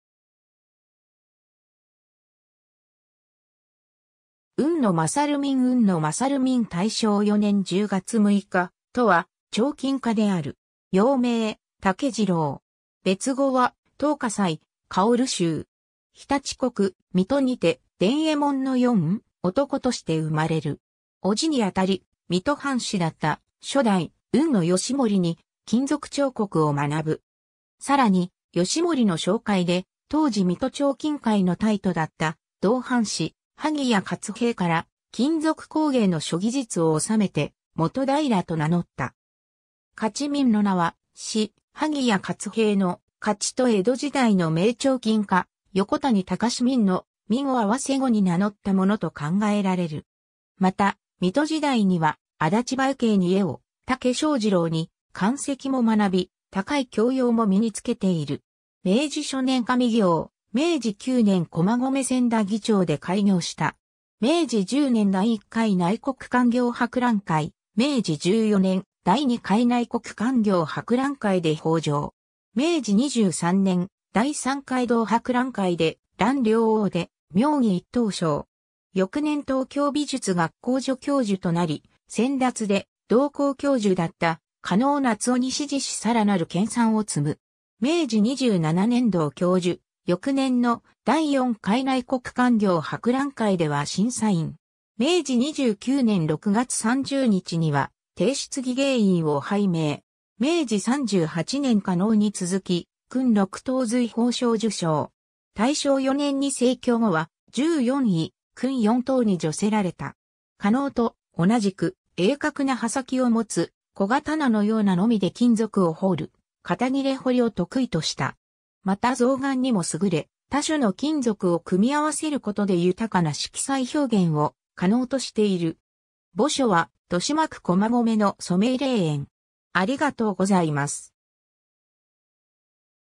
海野勝珉
海野勝珉 海野 勝珉（うんの しょうみん、天保15年5月15日（1844年6月30日） - 大正4年（1915年）10月6日）とは、彫金家である。
幼名・竹次郎。
別号は東華斎、芳州。
 常陸国・水戸にて、伝右衛門の四男として生まれる。
叔父にあたり水戸藩士だった初代・海野美盛に金属彫刻を学ぶ。
更に美盛の紹介で、当時水戸彫金界の泰斗だった同藩士・萩谷勝平から金属工芸の諸技術を収めて、基平と名乗った。
「勝珉」の名は、師・萩谷勝平の「勝」と江戸時代の名彫金家・横谷宗珉の「珉」を合わせ後に名乗ったものと考えられる。
また水戸時代には、安達梅渓に絵を、武庄次郎に漢籍も学び、高い教養も身につけている。
 明治初年上京、明治9年（1876年）駒込千駄木町で開業した。
明治10年（1877年）第1回内国勧業博覧会、明治14年（1881年）第2回内国勧業博覧会で褒状。
明治23年（1890年）、第3回同博覧会で「蘭陵王」で妙技一等賞。
翌年東京美術学校助教授となり、先達で同校教授だった加納夏雄に師事し更なる研鑽を積む。
明治27年（1894年）同教授、翌年の第4回内国勧業博覧会では審査員。
明治29年（1896年）6月30日には帝室技芸員を拝命。
明治38年（1905年）加納に続き勲六等瑞宝章受章。
大正4年に逝去後は、従四位勲四等に叙せられた。
 加納と同じく、鋭角な刃先を持つ小刀のような鑿で金属を彫る片切彫を得意とした。
また象嵌にも優れ、多種の金属を組み合わせる事で豊かな色彩表現を可能としている。
 墓所は豊島区駒込の染井霊園。